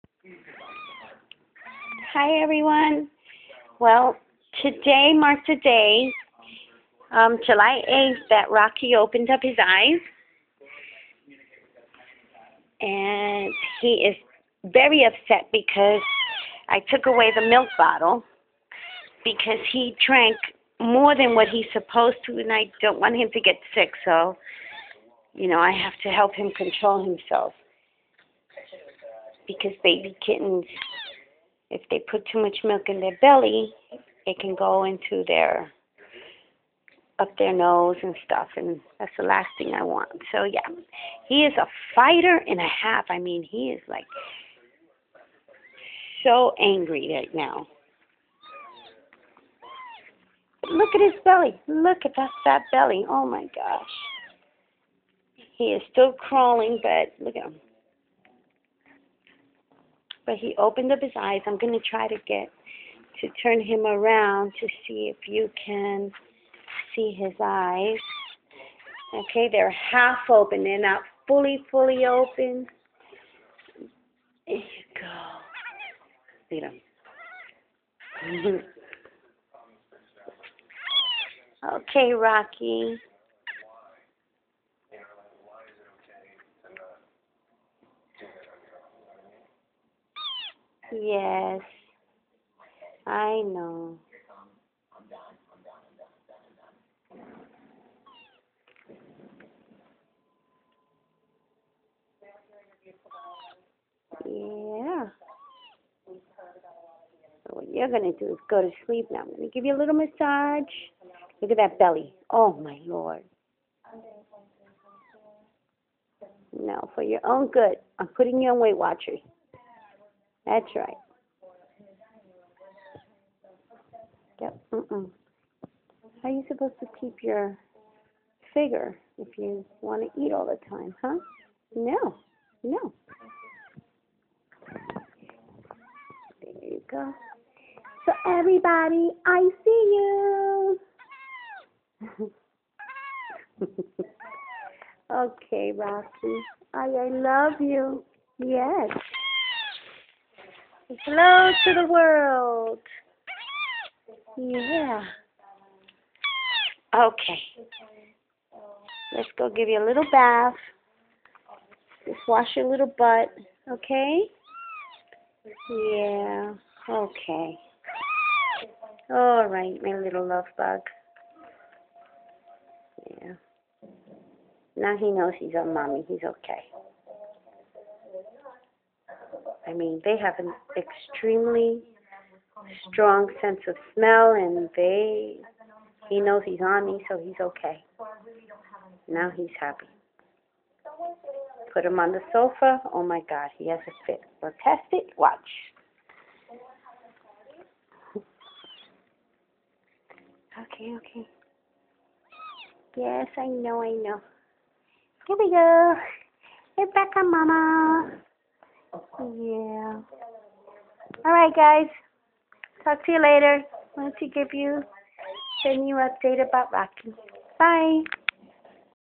Catbook Movie: Rocky opens his eyes
Rocky opens his eyes for the first time